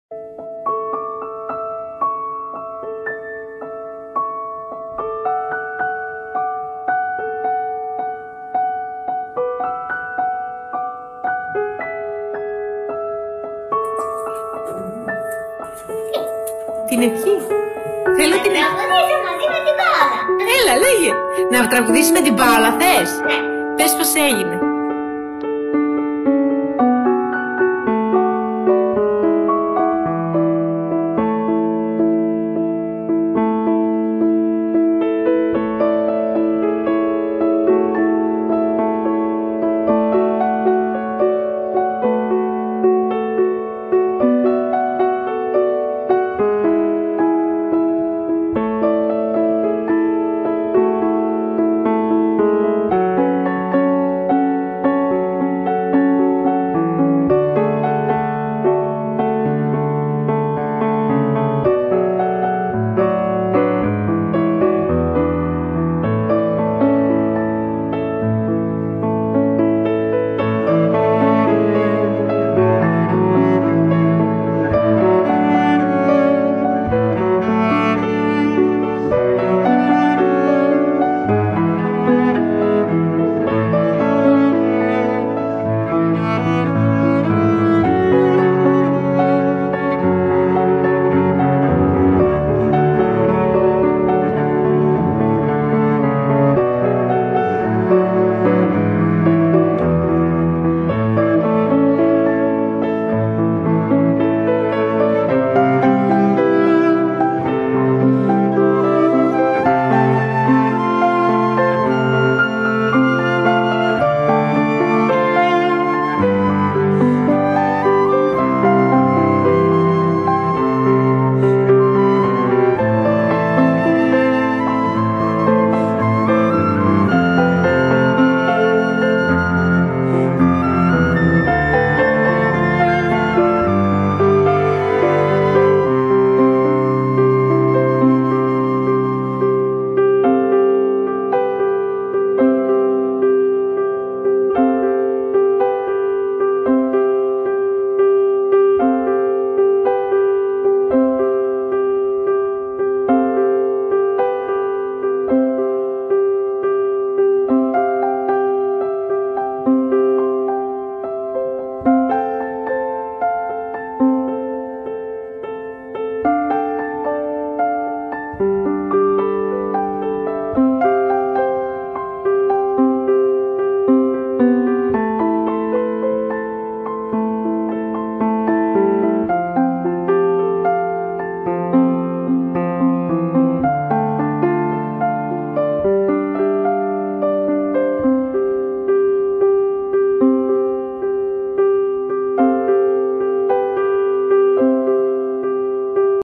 Η Πάολα πραγματοποίησε την ευχή της μικρής Παρασκευής!!
Η μικρή Παρασκευή είχε κάνει μια ευχή να τραγουδήσει με την Πάολα... λίγους μήνες μετά η ευχή της έγινε πραγματικότητα... :) Η Πάολα κατάφερε να πραγματοποιήσει την ευχή της μικρής Παρασκευής, χαρίζοντάς της χαρά και δείχνοντας πως άνθρωπος γεννιέσαι... δεν γίνεσαι!!

Εγώ δημιούργησα το βίντεο αυτό όχι για να κερδίσω κάτι.. αλλά για να δείξω πόση μεγάλη ψυχή έχει η Πάολα και πόσο άνθρωπος είναι.. με όλη την σημασία της λέξης. Την αγαπάω και την θαυμάζω και δεν θα σταματήσω ποτέ να νιώθω αυτά τα συναισθήματα για την Πάολα!!